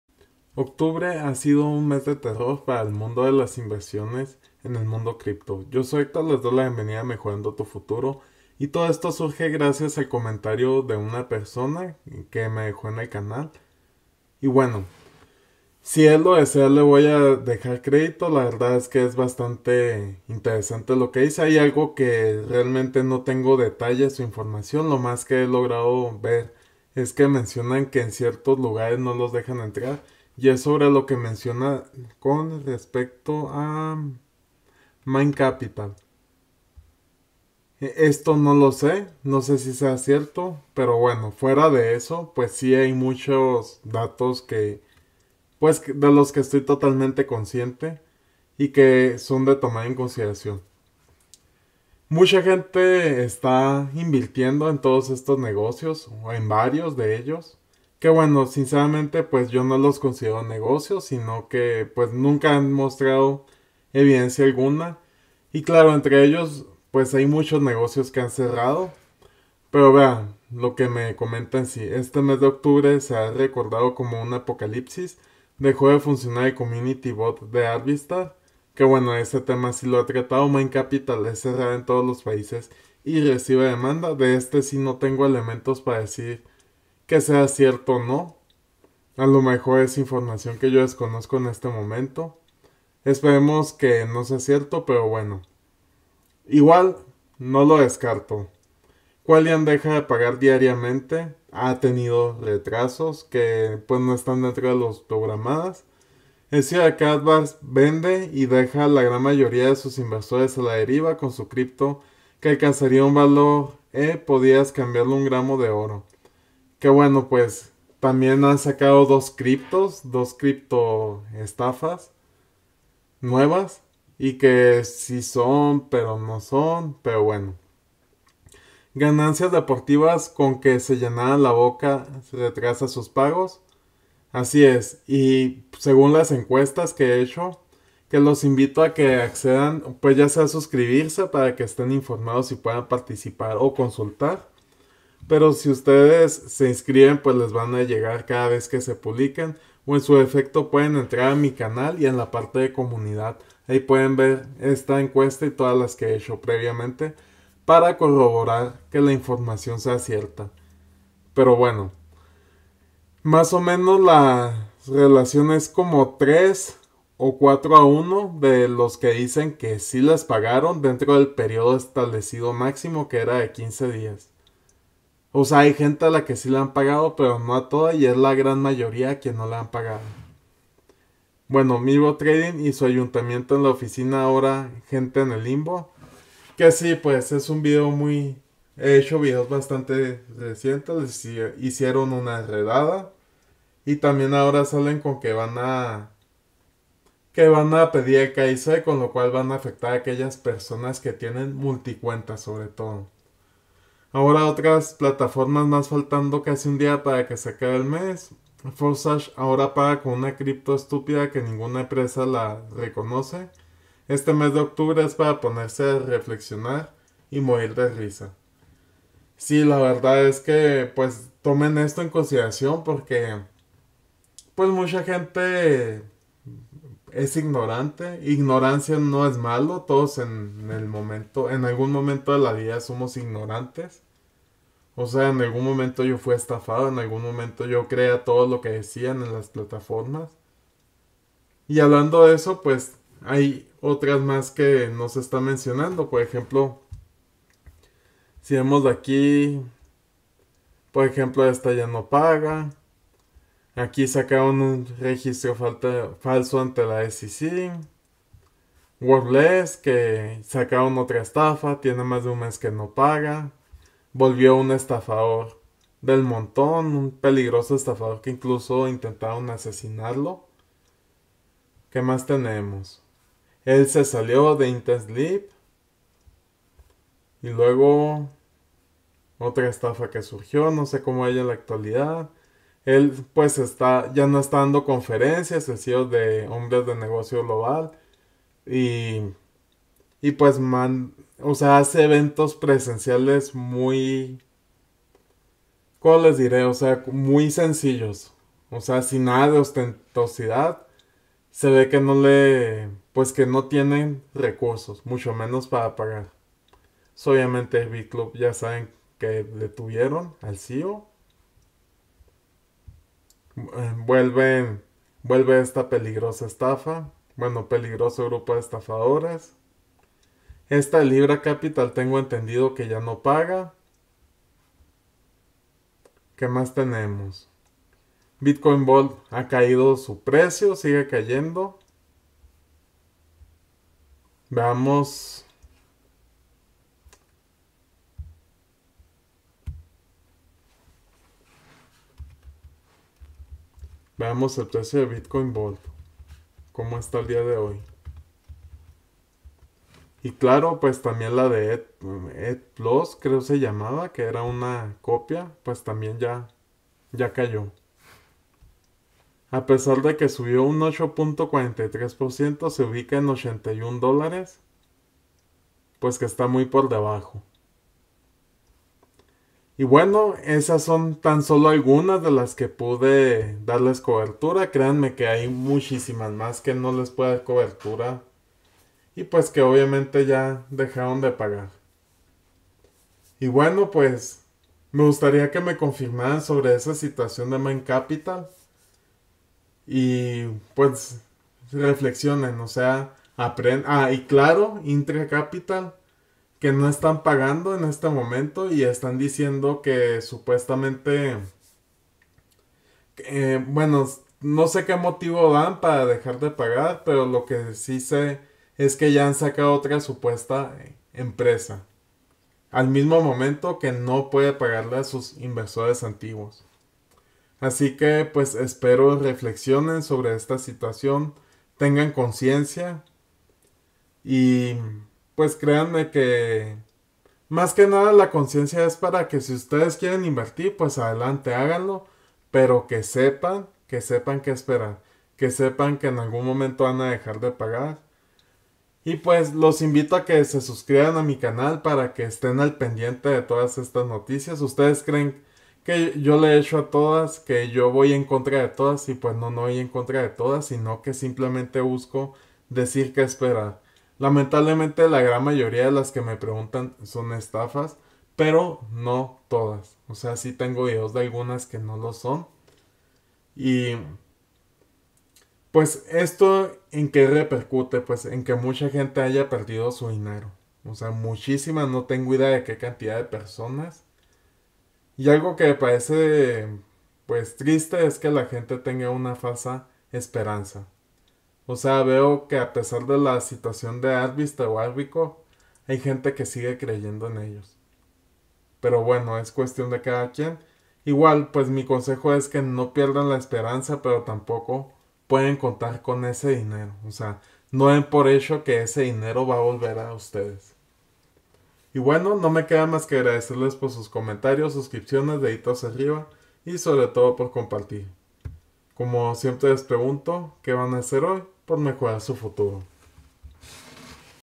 Mes de 👻Terror para Inversionistas y ¿😈Mind Capital🐀 Cierra🚫?
Un agradecimiento especial a por ser la fuente de inspiración del video

En esta ocasión les comparto algunos datos preocupantes sobre inversiones de este mes de octubre, te comparto todo lo que debes de saber

¿Más Inversiones?